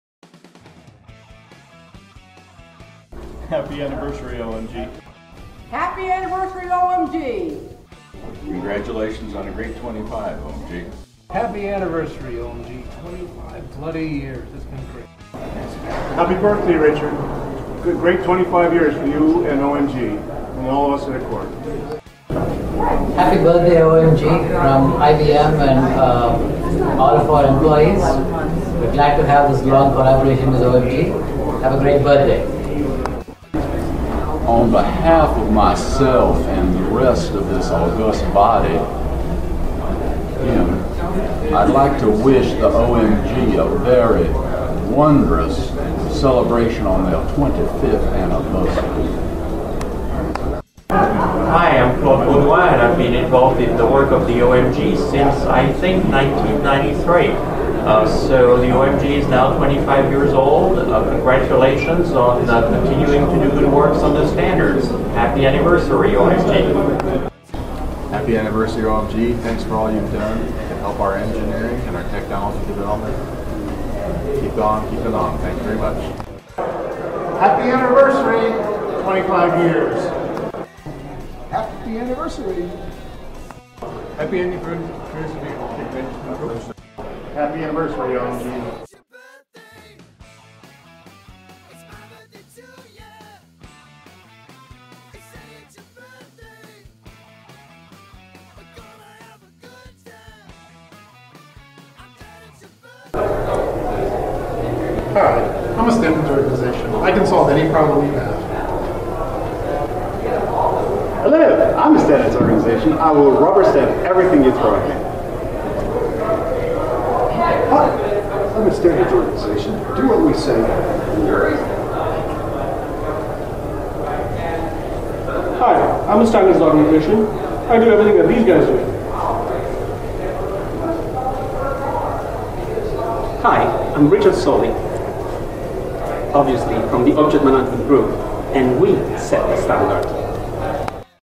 OMG 25th Anniversary Greetings
OMG is celebrating it's 25 anniversary this year! And our members couldn't be more excited for us!